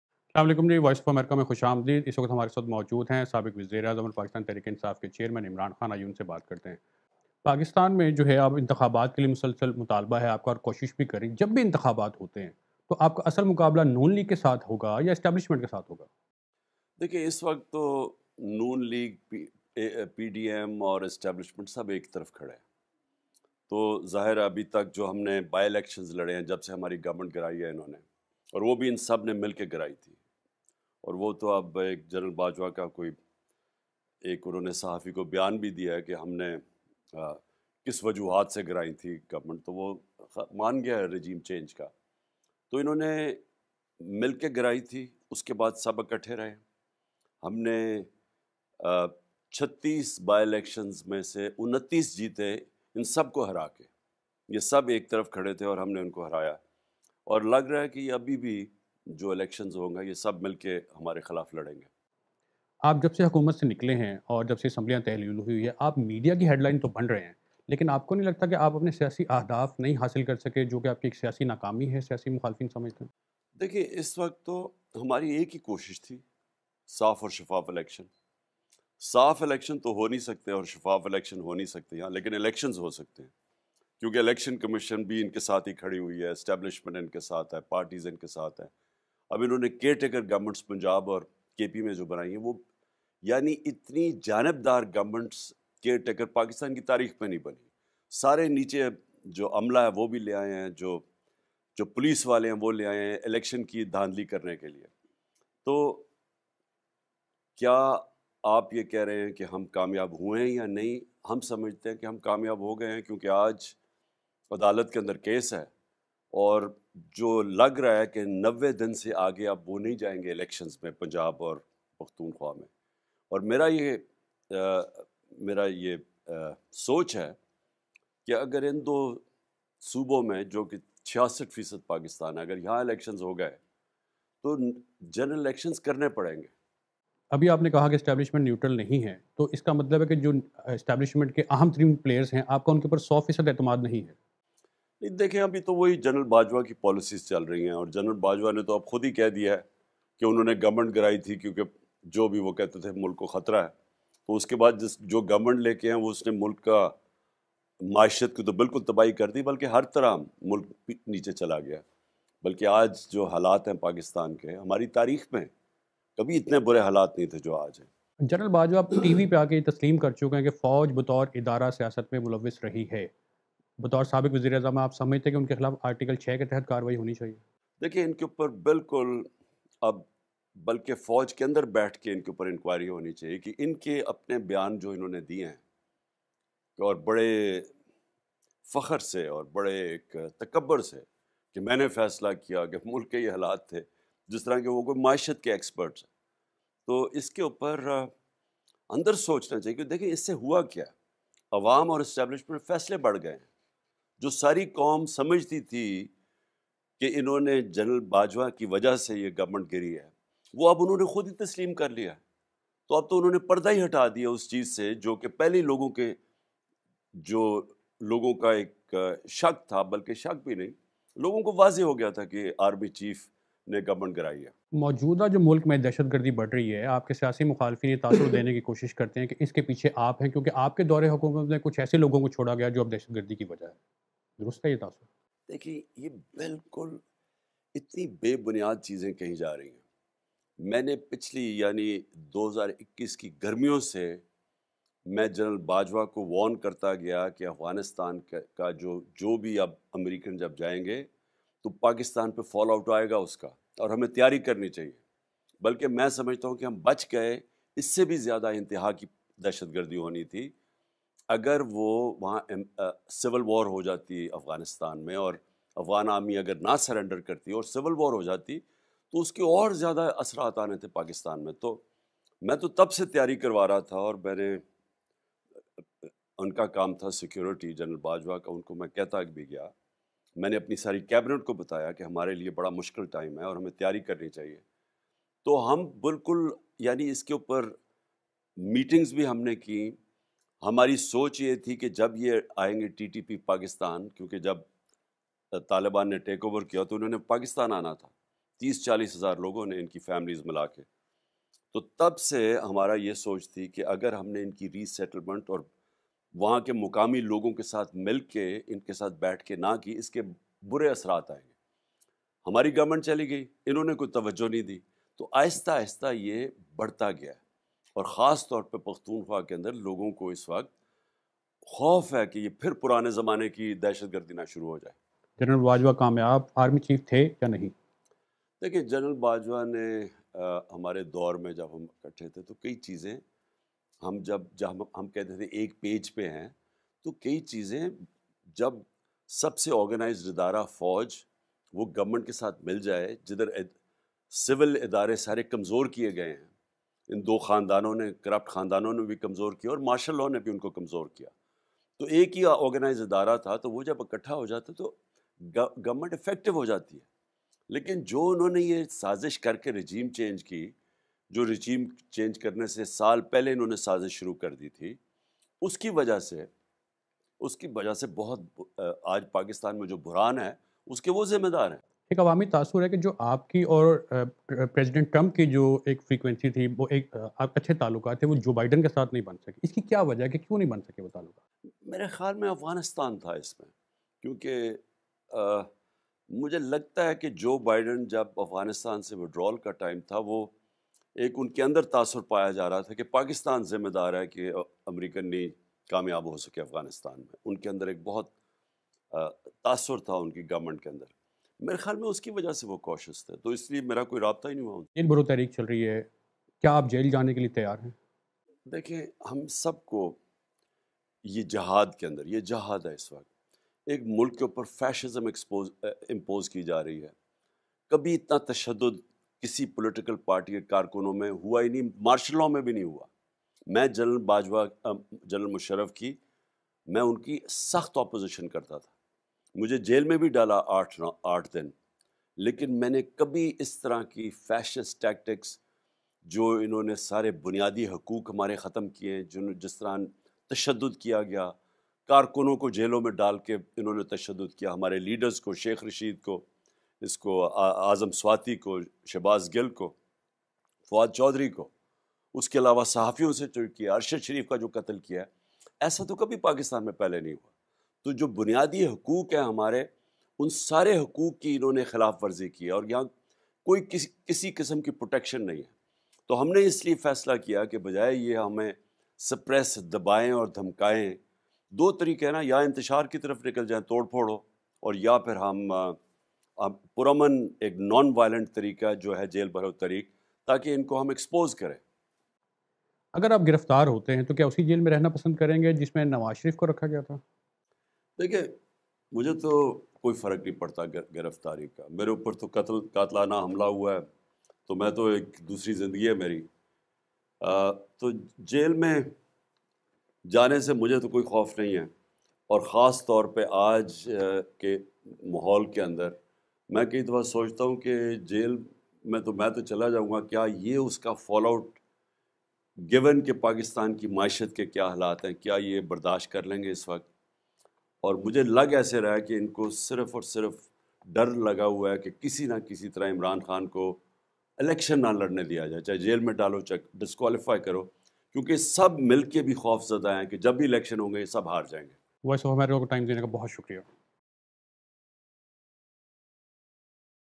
Chairman PTI Imran Khan Exclusive Interview on Voice of America with Zia-Ur Rahman
Chairman PTI Imran Khan Exclusive Interview on Voice of America with Zia-Ur Rahman (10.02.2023)